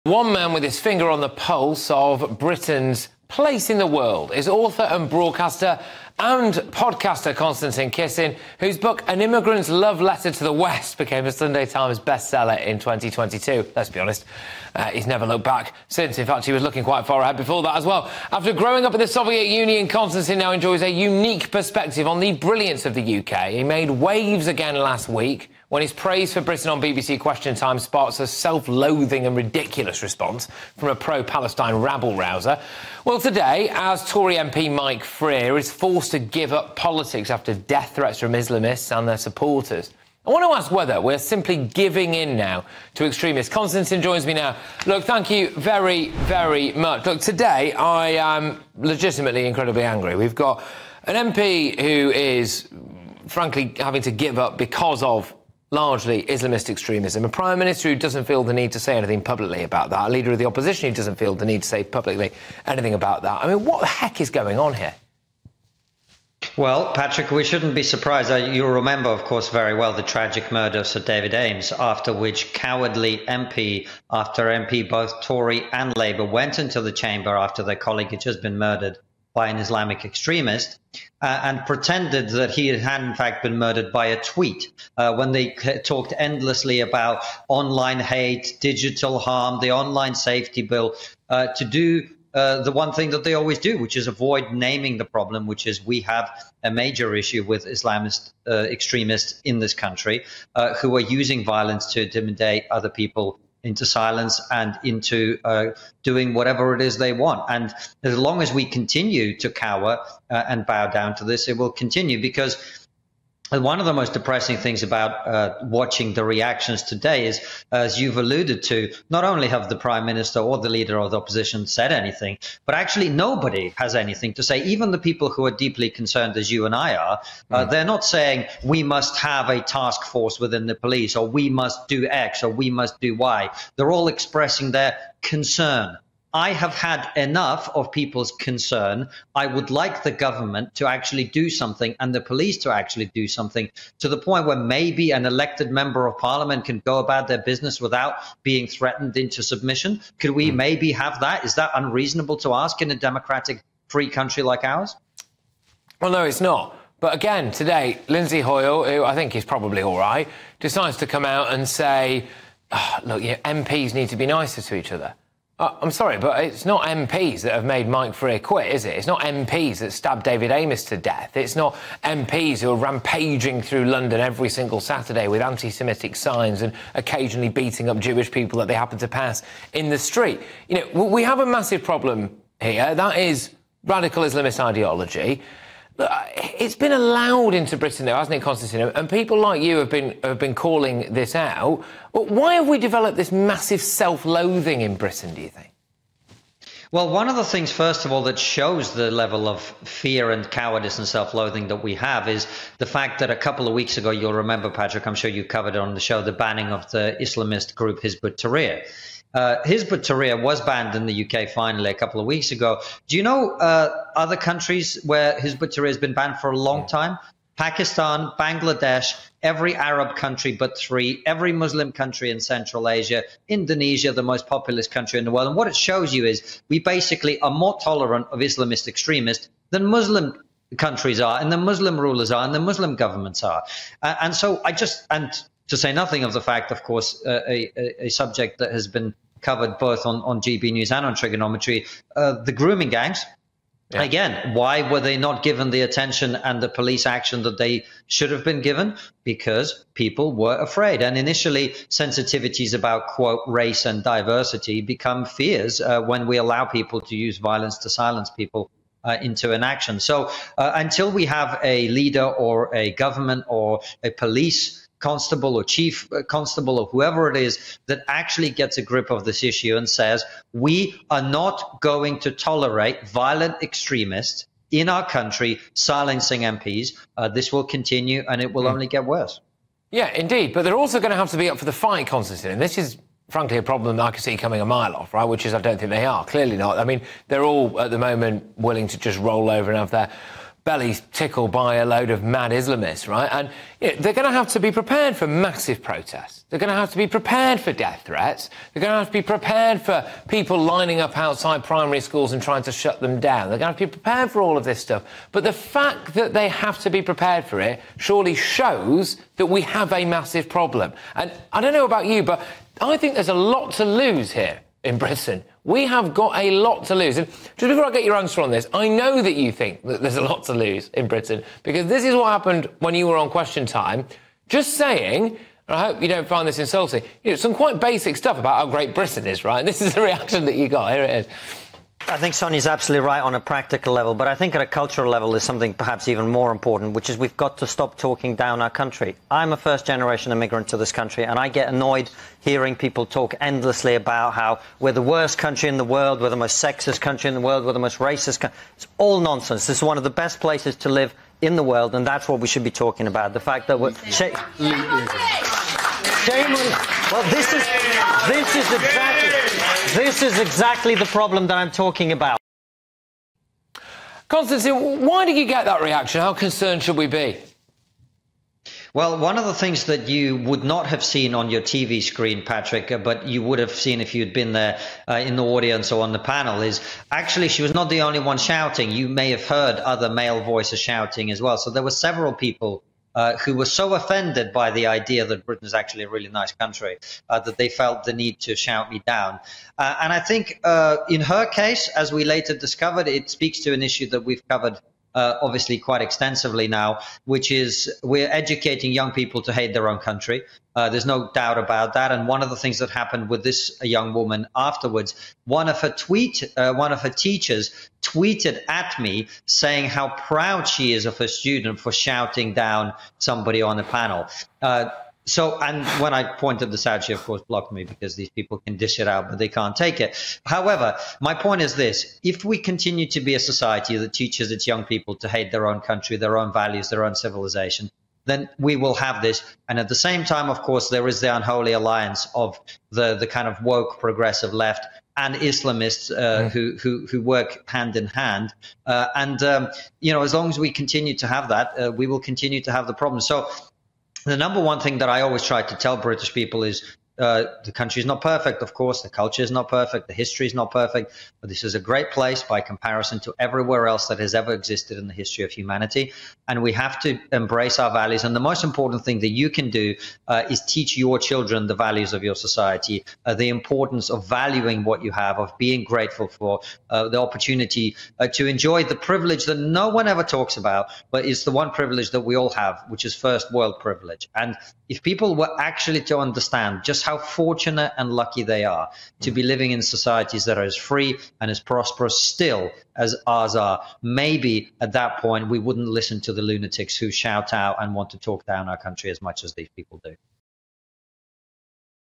'Britain is more tolerant of muslim extremists than most muslim countries' | Konstantin Kisin
‘I have had enough of peoples ‘concern’, and I’d like politicians and the police to actually do something about it’

Konstantin Kisin says politicians always avoid naming the problem which is 'we have a major problem with Islamist extremists in this country'.